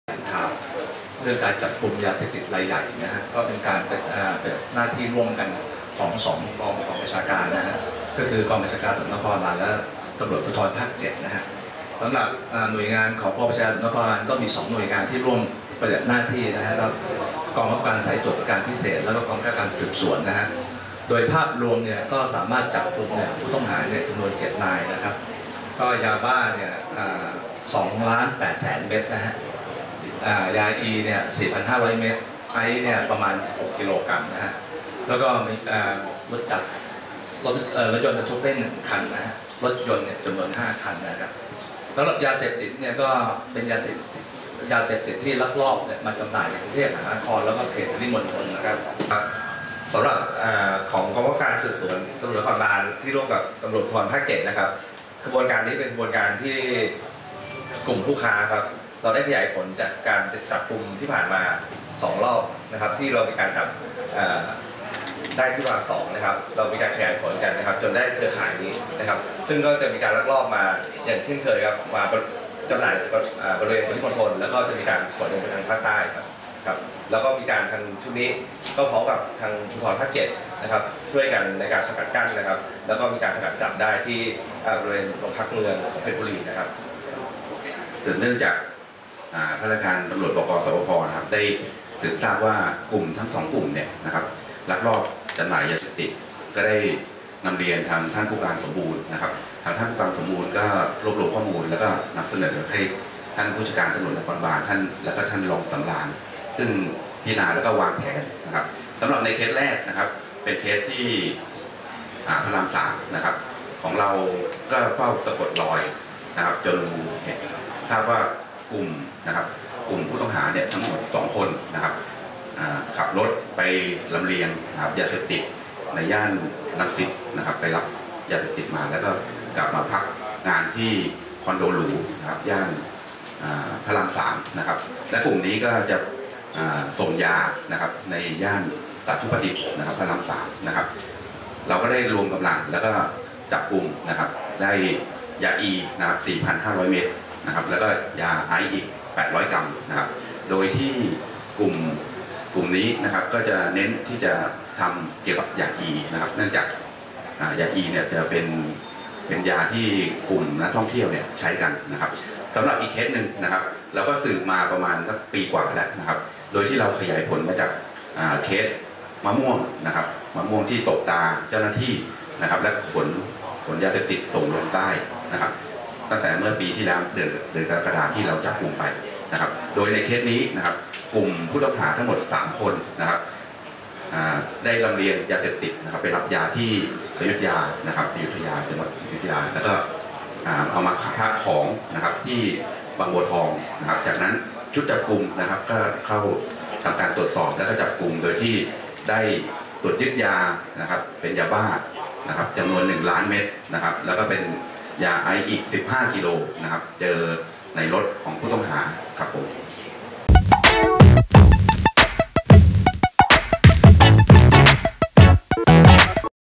ผบช.น. แถลงข่าวจับแก๊งเครือข่ายยาเสพติดรายใหญ่ ยาบ้า 2,800,000 เม็ด ไอซ์ 15 กก
2 พ.ย. 63 เวลา 11.30 น. พล.ต.ท.ภัคพงศ์ พงษ์เภตา ผบช.น. พร้อมด้วย พล.ต.ต.ปิยะ ต๊ะวิชัย รองผบช.น. , พล.ต.ต.สหรัฐ ศักดิ์ศิลปชัย รอง ผบช.น. , พล.ต.ต.สำราญ นวลมา รอง ผบช.น. , พล.ต.ต.สมบูรณ์ เทียนขาว ผบก.สปพ. , พ.ต.อ.ธีระชัย ชำนาญหมอ รอง ผบก.สส.บช.น. และเจ้าหน้าที่ที่เกี่ยวข้อง
ร่วมแถลงข่าว การจับกุมคดีแก๊งเครือข่ายยาเสพติดรายใหญ่ในพื้นที่ กรุงเทพฯและปริมณฑล ของกลางประกอบด้วย ยาบ้า 2,800,000 เม็ด, ยาอี 4,580 เม็ด, ยาคีตามีน 0.85 กรัม และยาไอซ์ 15 กิโลกรัม ณ ลานแถลงข่าว ชั้น 1 อาคาร บช.น.